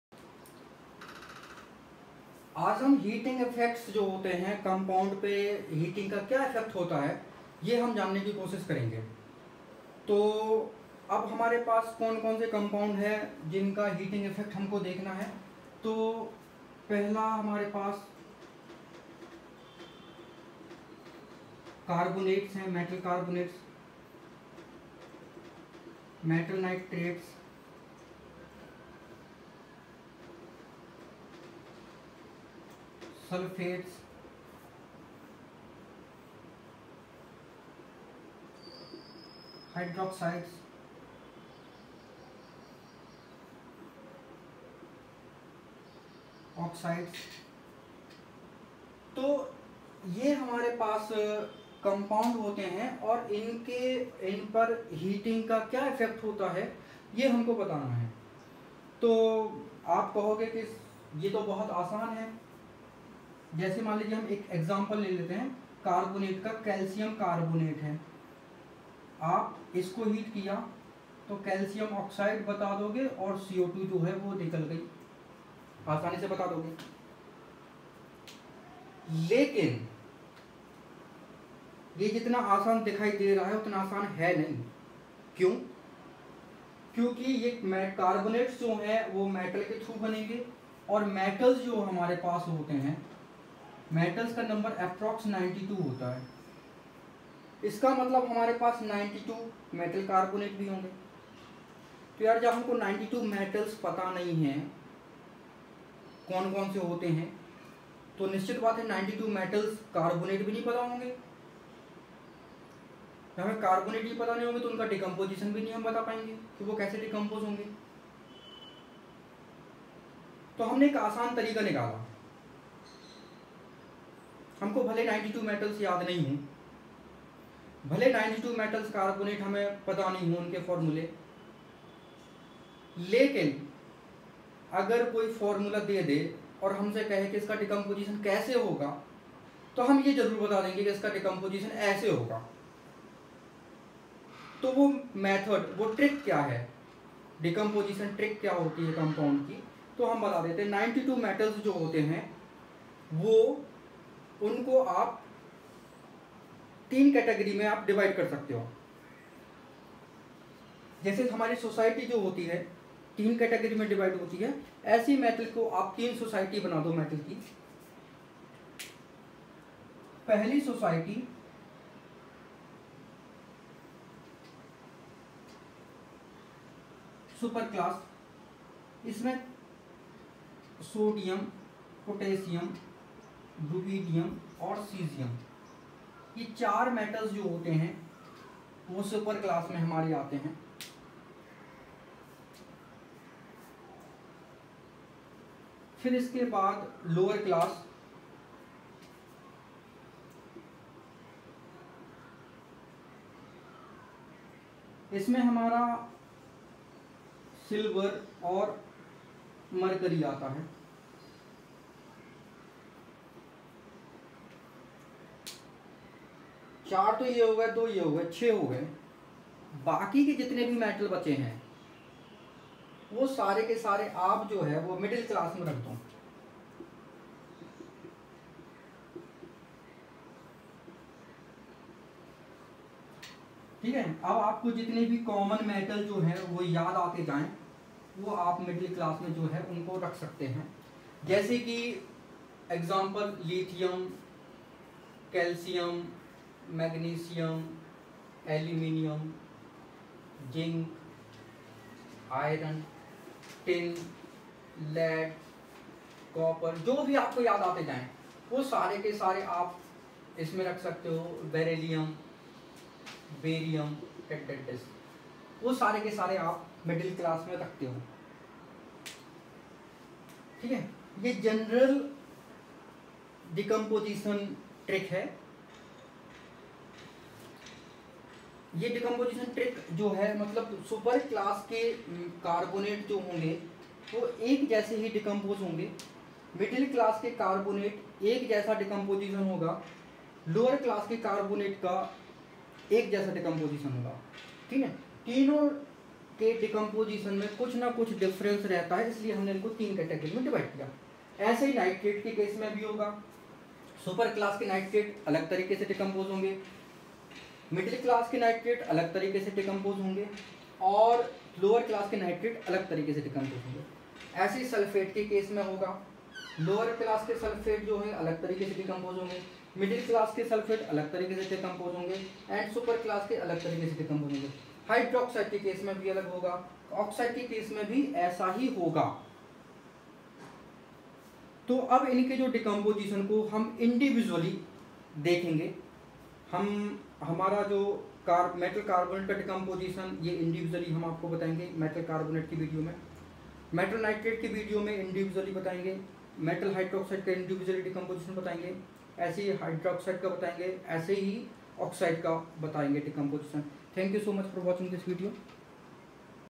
EasyTrick to remember heating effects on salts
Trick to remember decomposition of metal ( Carbonates, Sulphates, Nitrates, Hydroxides, Oxides)